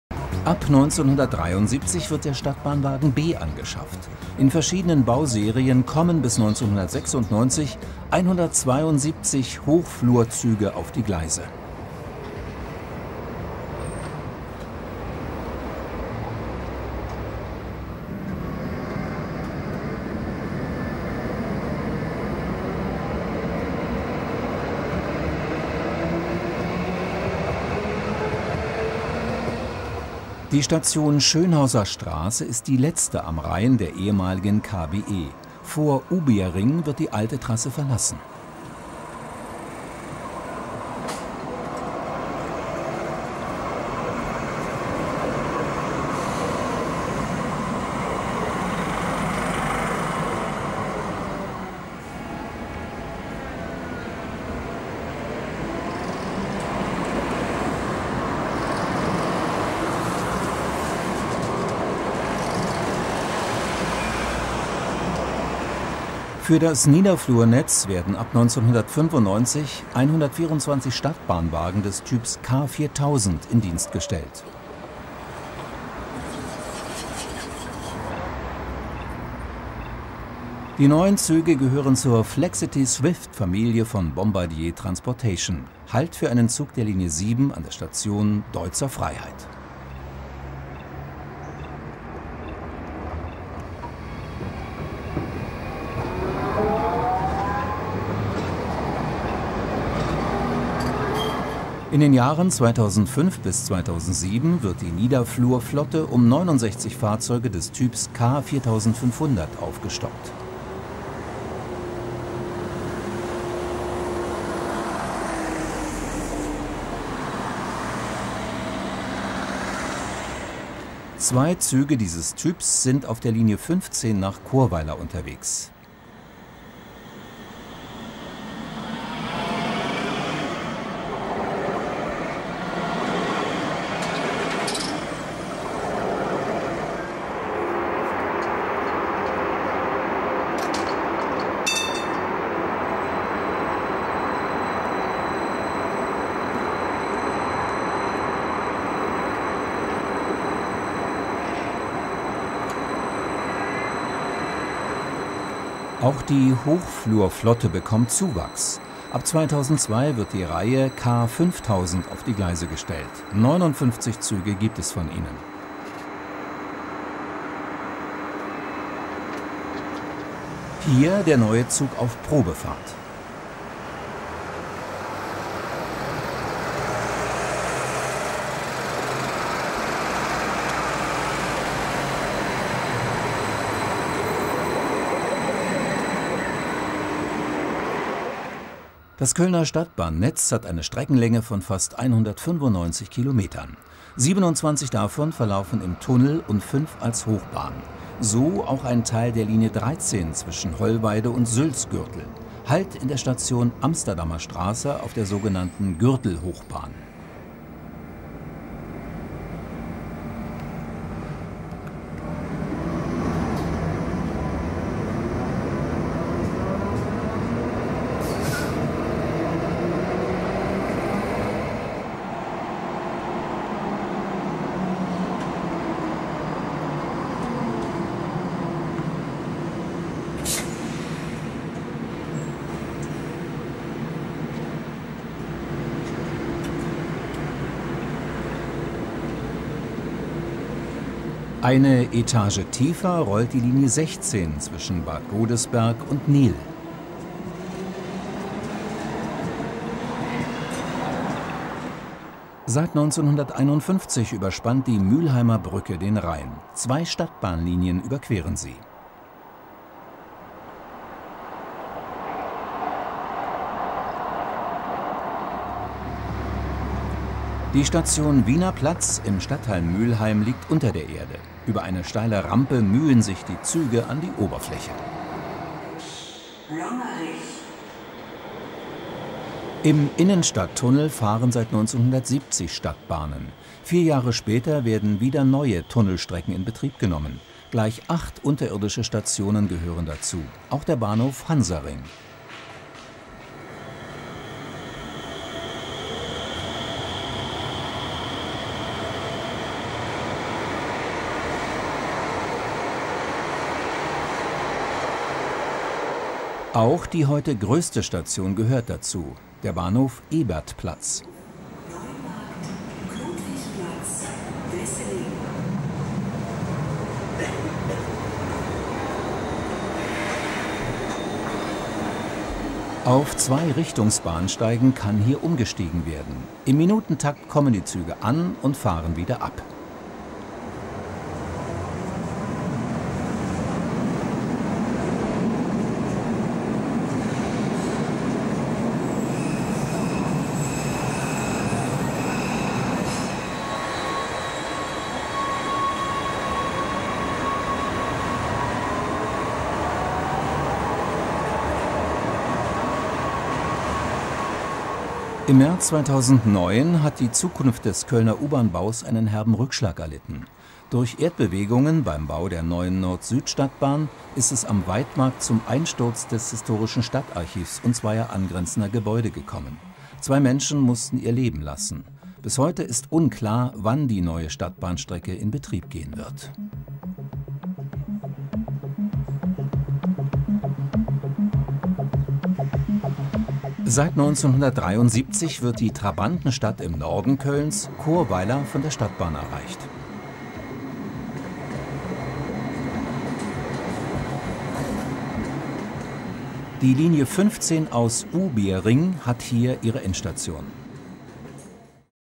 Stadtbahn Köln
Die Stadtbahn Köln ist das Rückgrat des öffentlichen Nahverkehrs. Zwölf Linien befahren ein Streckennetz von fast 200 Kilometern. Außerdem ist die Kölner Stadtbahn über die Eisenbahnstrecken der HGK mit der Bonner Stadtbahn verknüpft.  Wir haben uns die Kölner Fahrzeuge mal angesehen. Viel Spaß beim Anschauen. Hier mehr
Für nur 2,99/Monat Kanalmitglied werden und inzwischen über 60 Filme in voller Länge und ohne Werbung Anschauen.